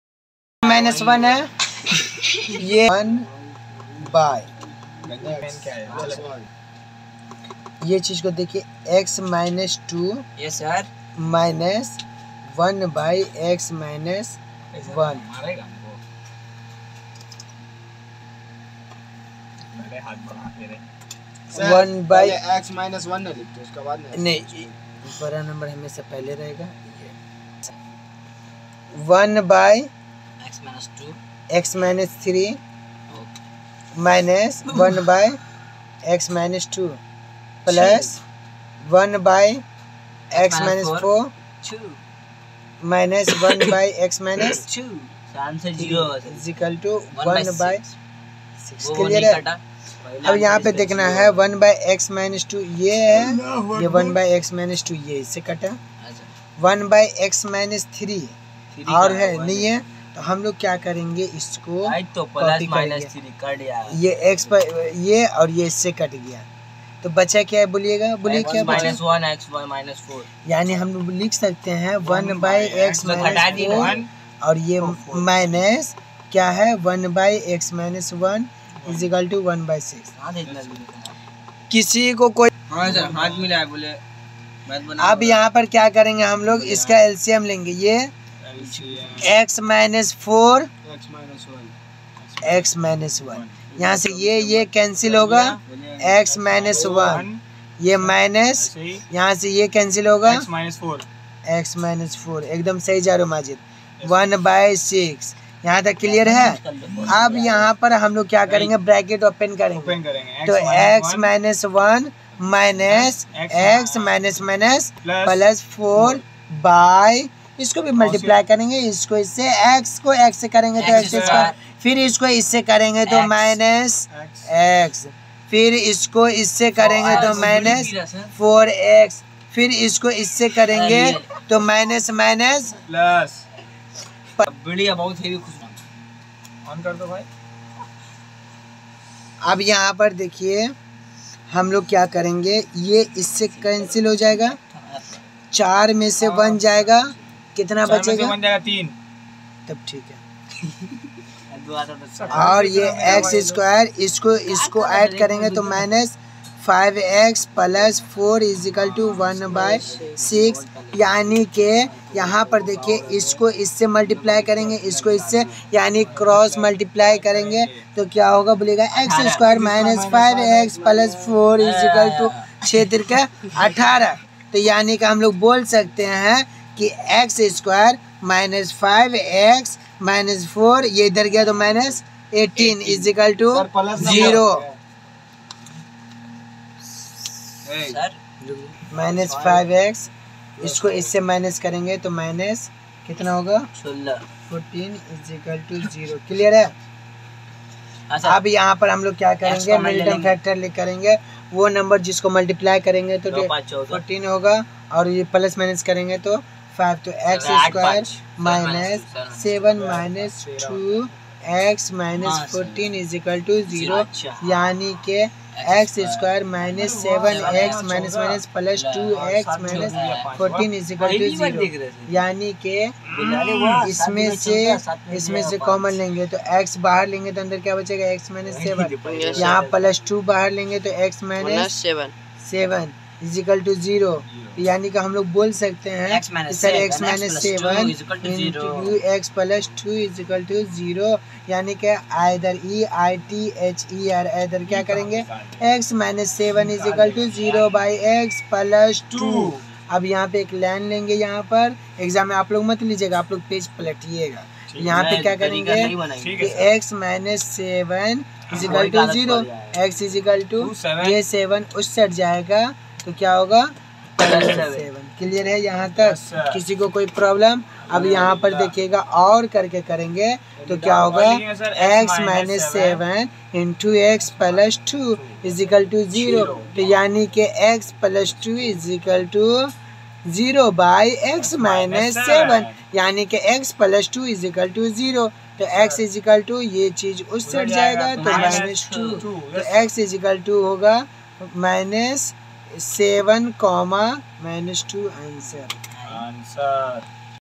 Quadratic equation variable | Q - 4(XIX) | Ex -1.2 | chapter -1 | class X WBBSE quadratic #10
quadratic equation variable for class X (10) solved (4.xix)questions | chapter1 | WBBSE | algebra,